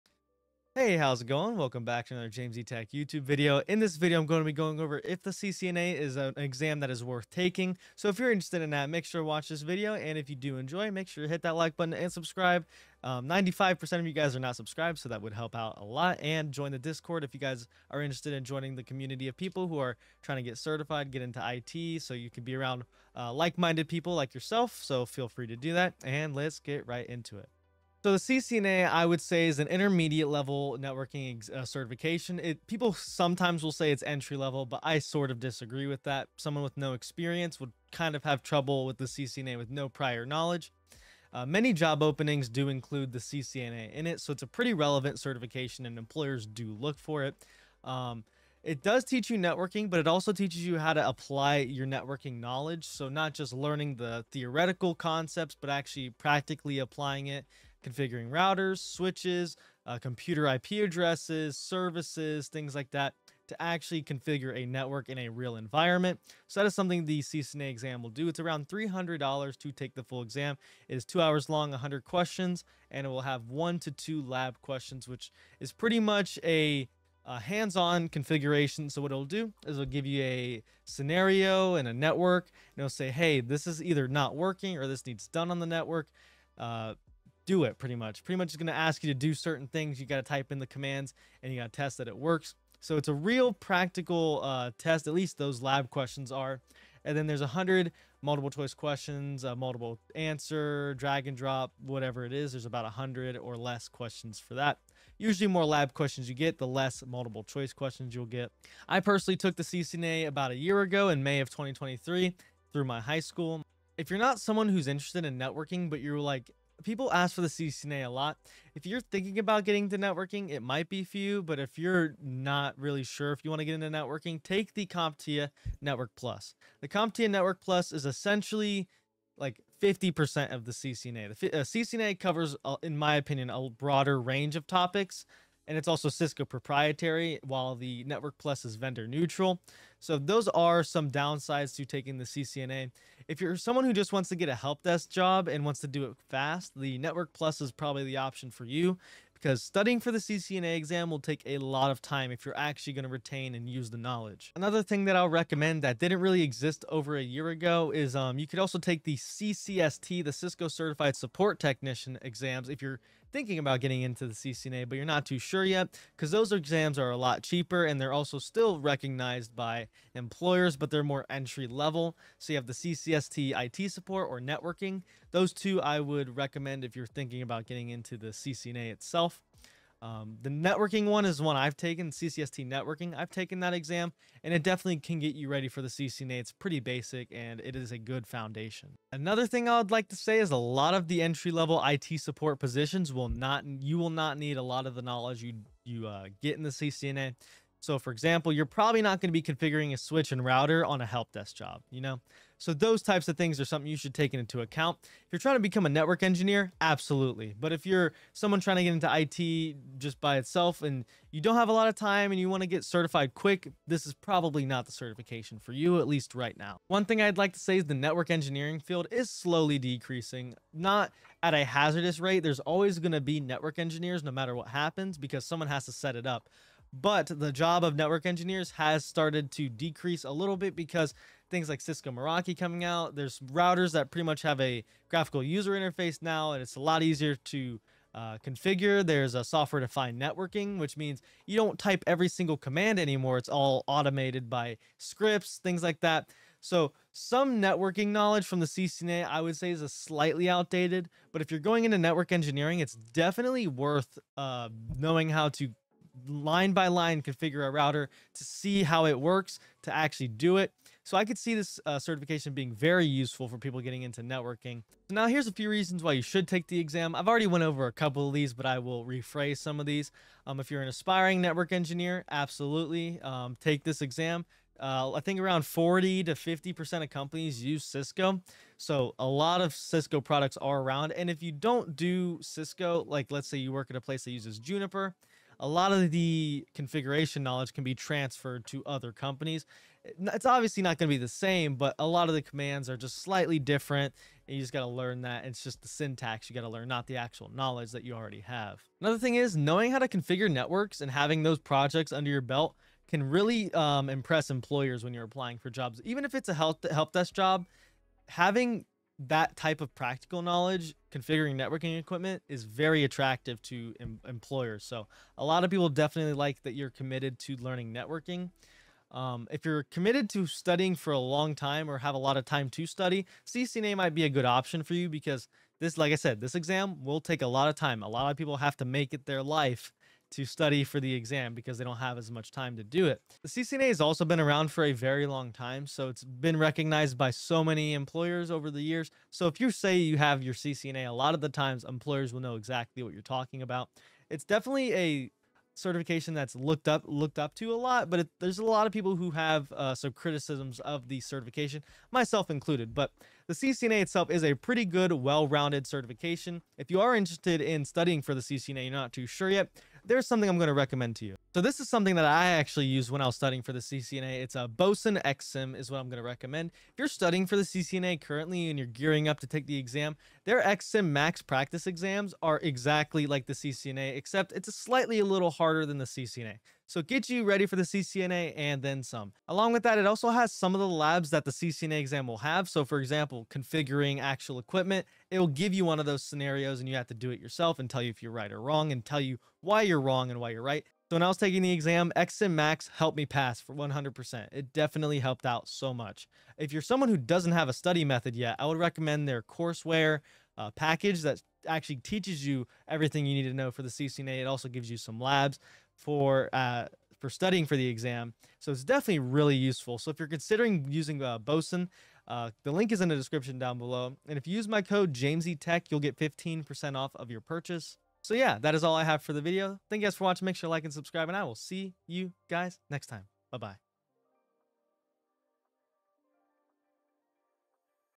Is The CCNA Still Worth Obtaining in 2025?
Hello! This video will go over the CCNA exam and if it is still worth obtaining. I'll be showing the pros and cons of studying and taking the exam to help you decide if it's a certification worth getting.